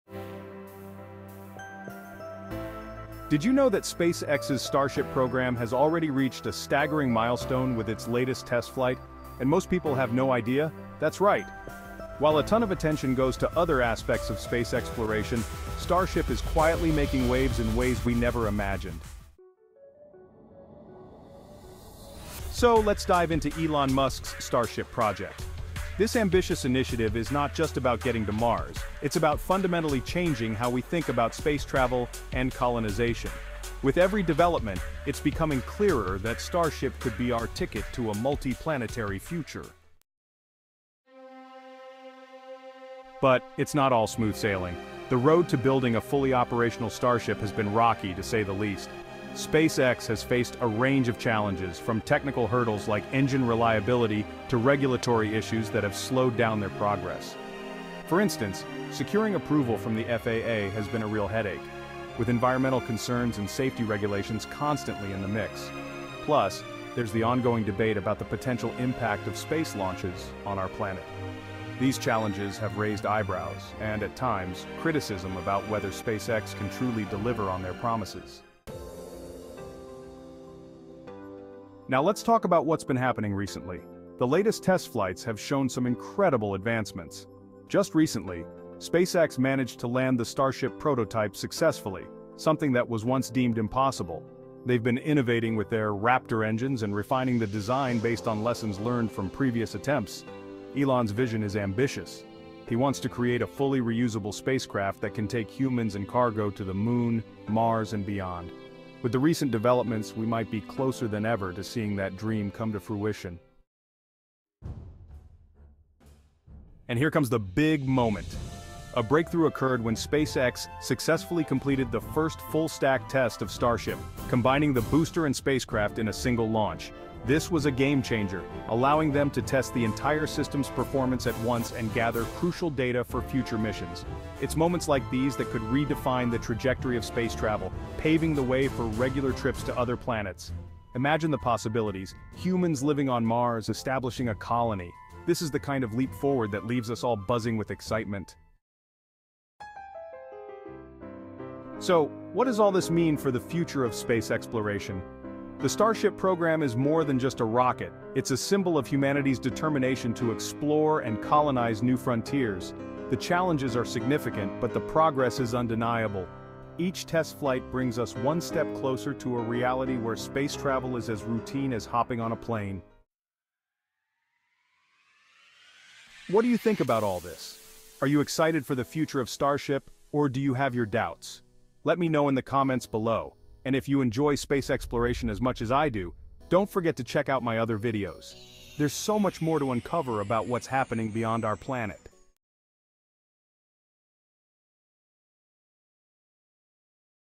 Elon Musk Prepares for the New Starship Launch: Are We Ready for Liftoff?
The countdown has begun! Elon Musk and SpaceX are preparing to launch the new Starship, marking a pivotal step in space travel and humanity’s journey to Mars. In this video, we’ll dive into what makes this Starship unique, the technology behind it, and the launch’s goals. With new features and ambitious plans, this launch could reshape our future in space. Join us as we explore SpaceX’s latest leap towards interplanetary exploration and beyond!